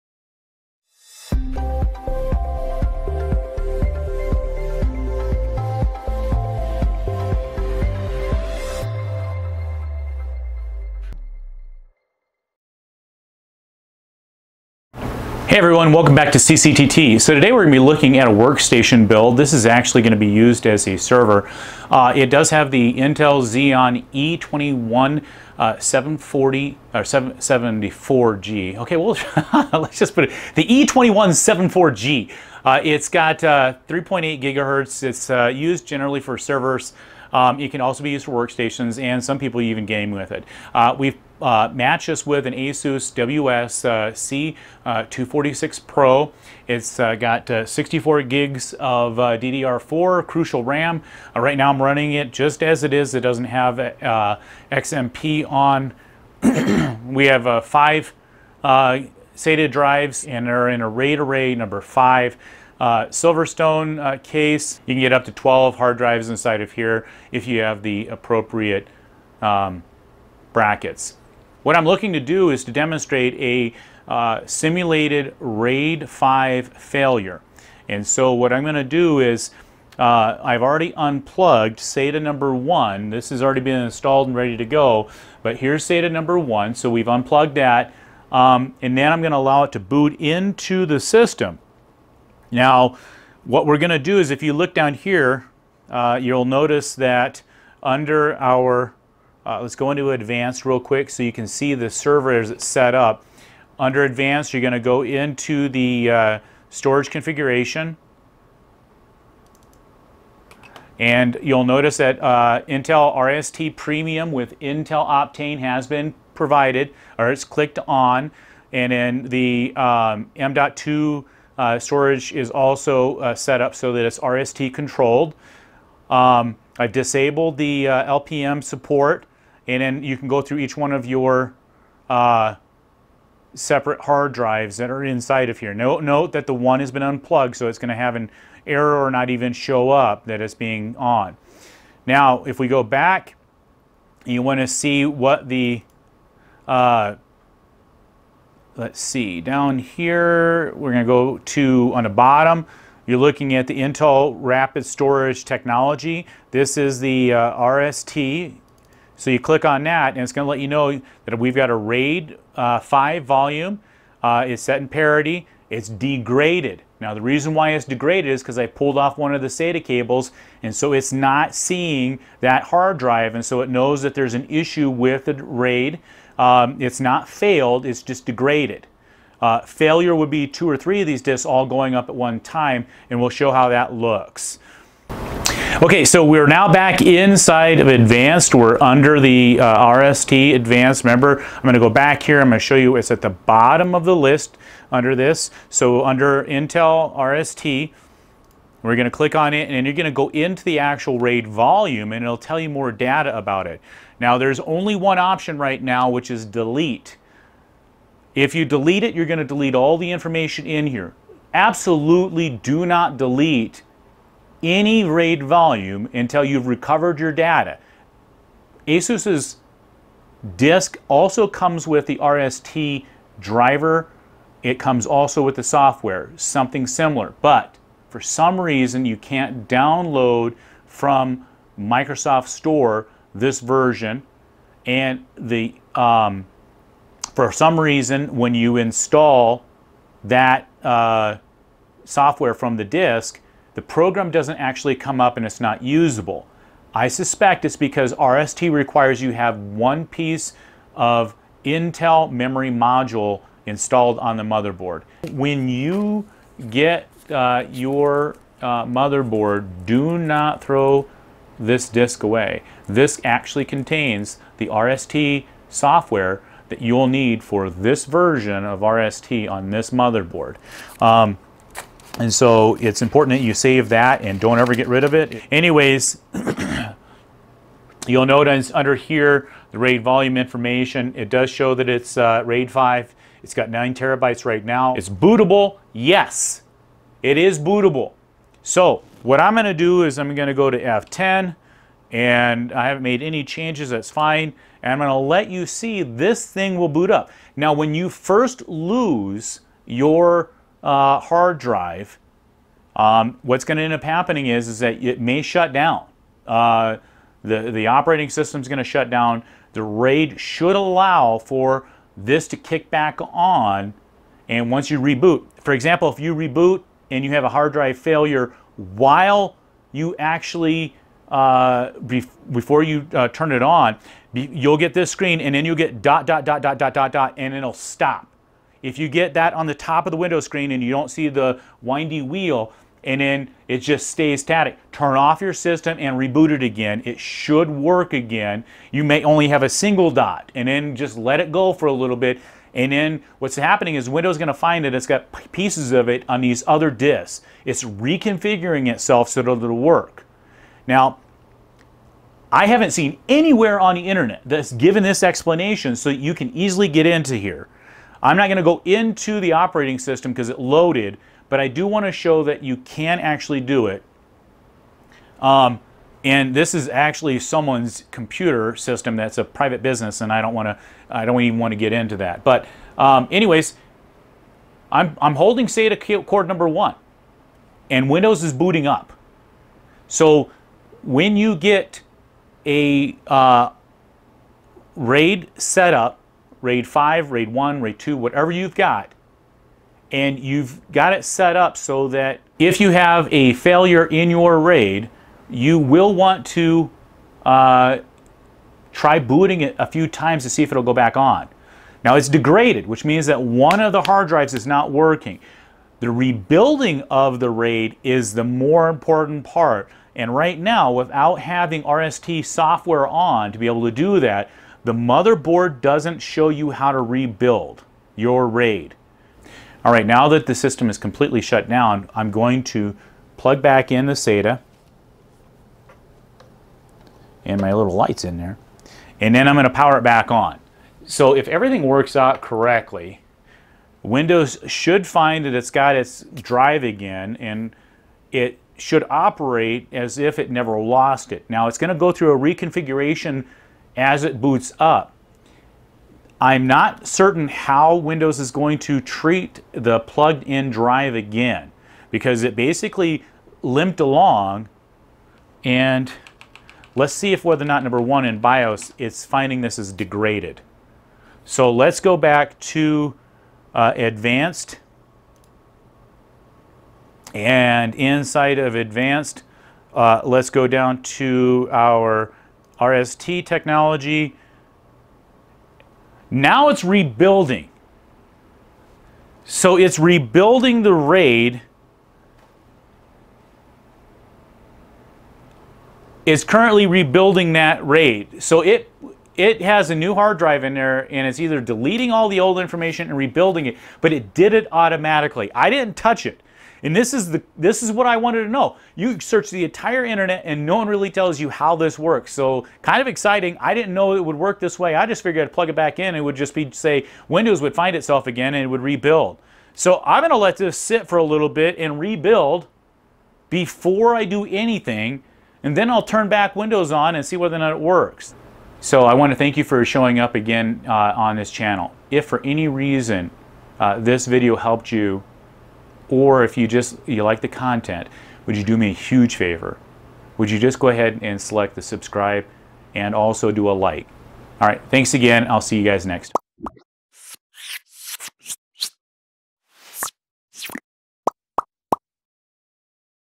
My RAID DeGrade!
Search as you will on the internet there isn't any good information on how to simulate a raid 5 failure on newer motherboards where the end user creates a Raid using Intel RST.  I walk you through a simulated raid degrade and show what happens and how to get back up and running without deleting your raid array.  This simulation is on a Microsoft Server 2022 machine powered by the Intel Xeon E-2174G and an ASUS WS C246 Pro.  Raid array was created in the ASUS bios and in the Silvertone case.